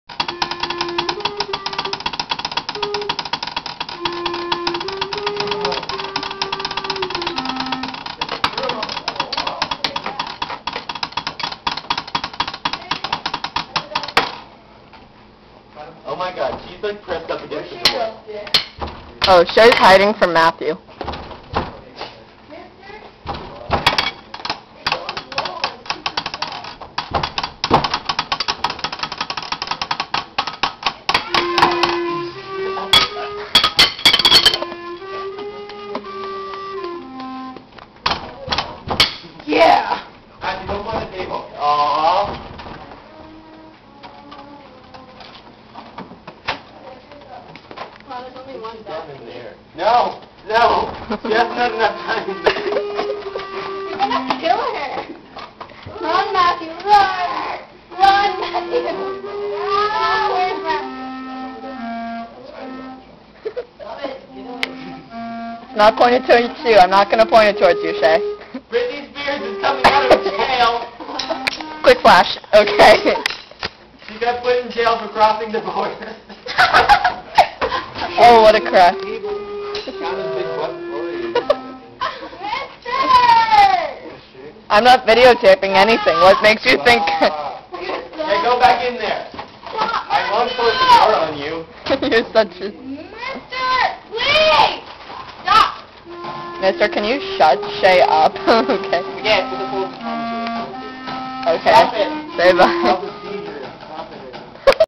The Band Room Indie Film
so Michelle wanted me to film her sad life in the band room..until Matthew stepped in and tried to be the paparazzi